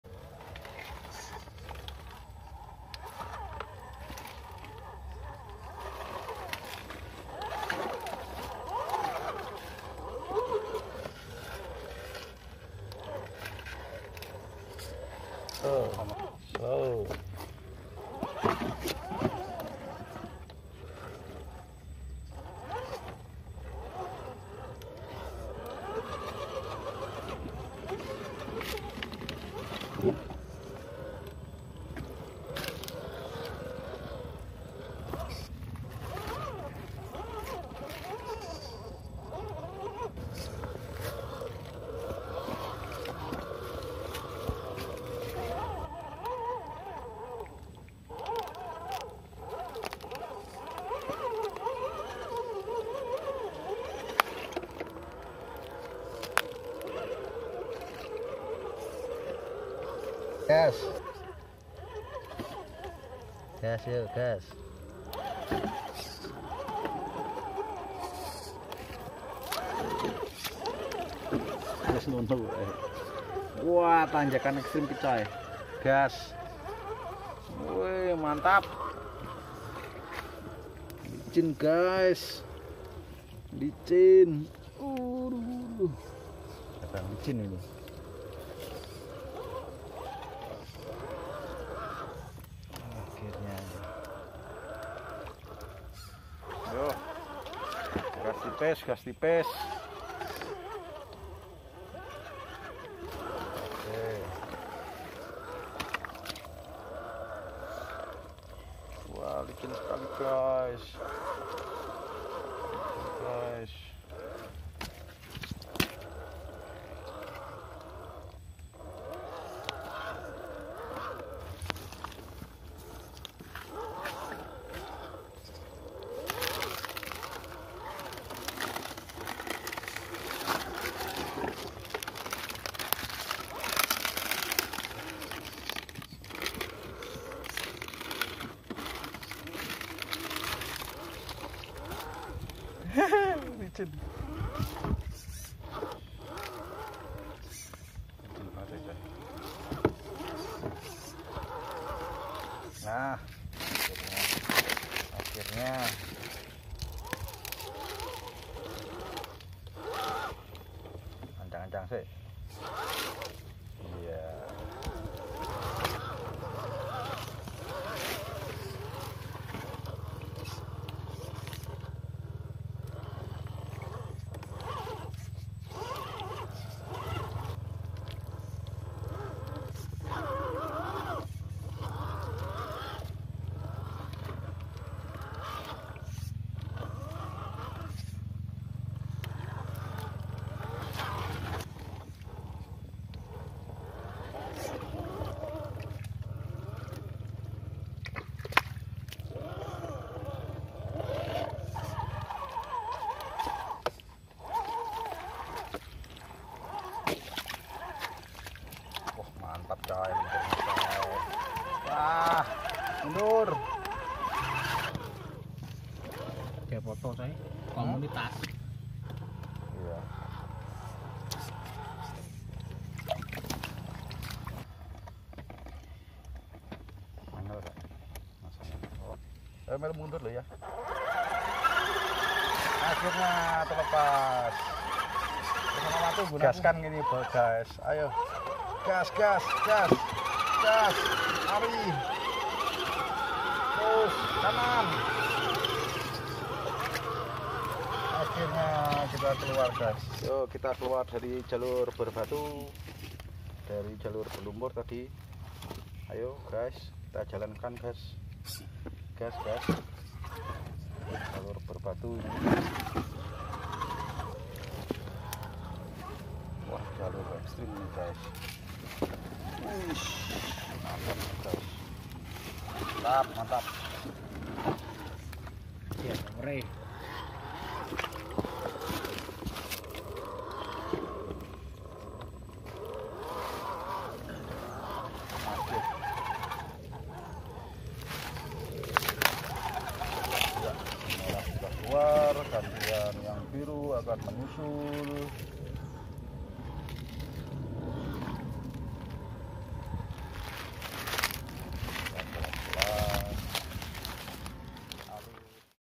1/10 Scale RC Car : RGT EX86100 V2 Through Muddy Tracks and Slippery Slopes
1/10 Scale RC Car : RGT EX86100 V2 Through Muddy Tracks and Slippery Slopes

Petualangan offroad menggunakan rc 1/10 RGT EX86100 V2  melalui jalur berlumpur dan tanjakan licin

Offroad adventure using the 1/10 RGT EX86100 V2 rc through muddy tracks and slippery slopes